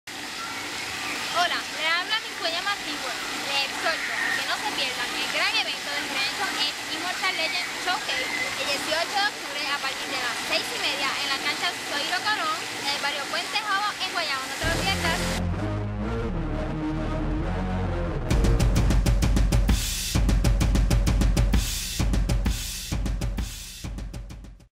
Ismarylis Velazquez - Immortal legends Show case ( Generation X )
Miss Teen World Guayama Joven talent de aqui dise presente al evento q hara historia en Guayama !! en la cancha bajo techo zoilo Colon de pnt Jobos